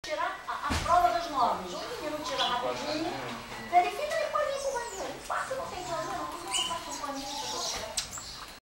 Hummingbird feeding station
Hummingbird (and some other bird) helping themselves at the feeding station of the Pousada Pedacinho de Céu in Ilha Grande, Brazil